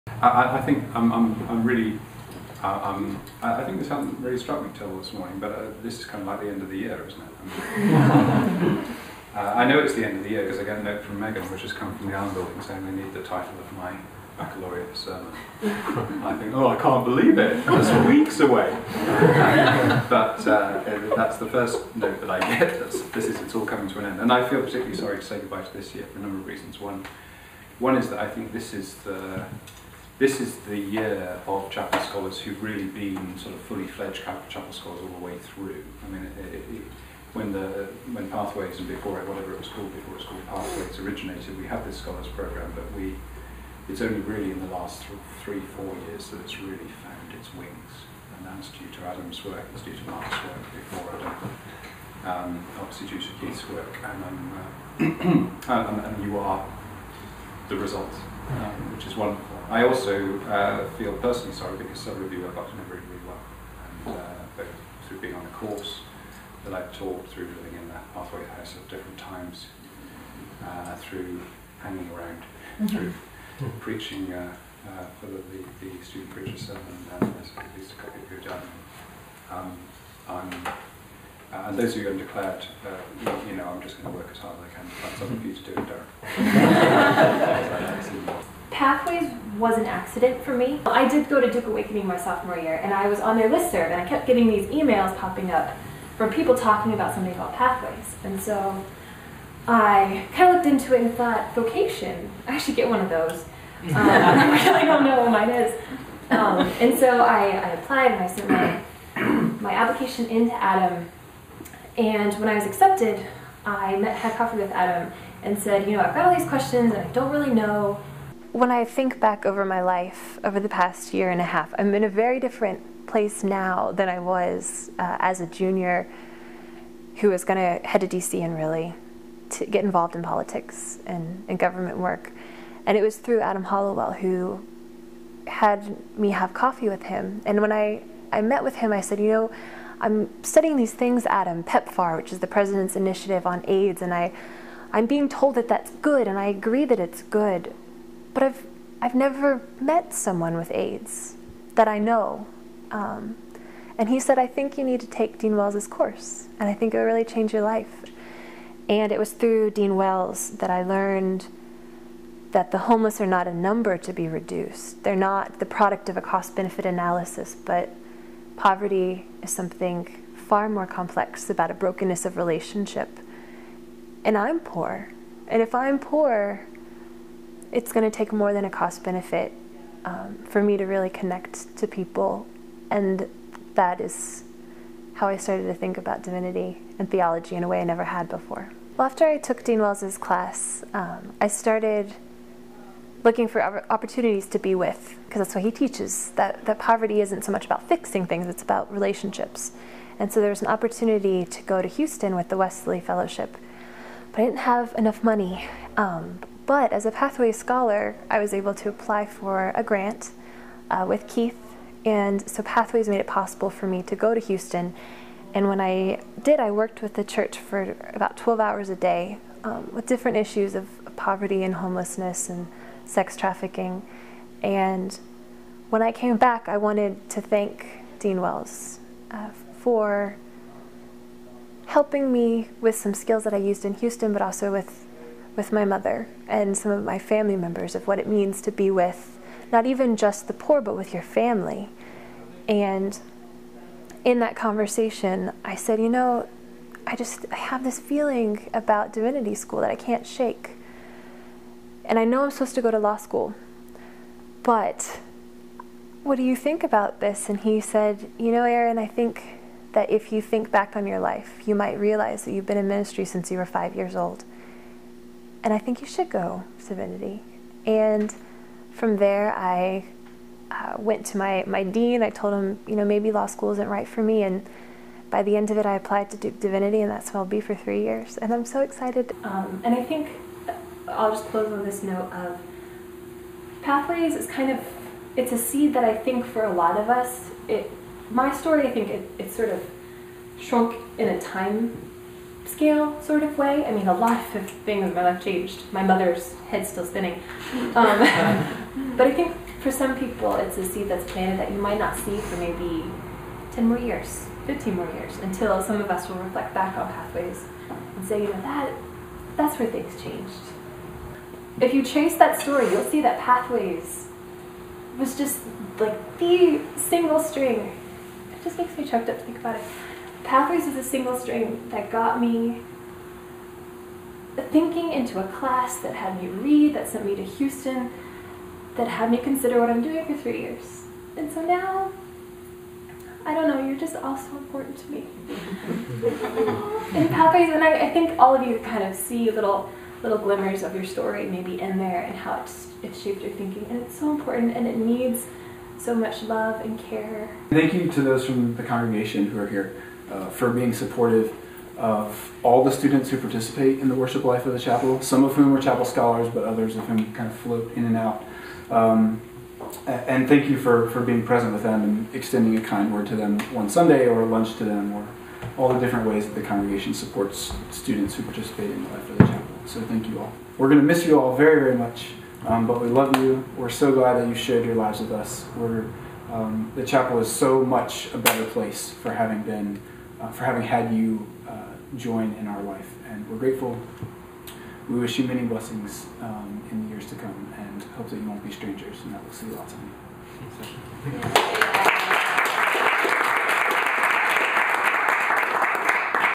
Duke Chapel Pathways 2011: Meet Erin Cloninger
Dean of the Chapel Sam Wells, Director of Student Ministry Adam Hollowell and  recent Duke Graduate and Pathways Scholar Erin Cloninger; video clips from May graduation of Pathways Scholars at the Wells home in Durham, NC. Conversation with Erin Cloninger about process of discernment in the call to ministry and decision to attend Duke Divinity School.